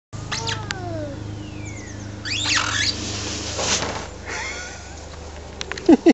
Not for the faint hearted
Zoe slipping down a slide in our back garden!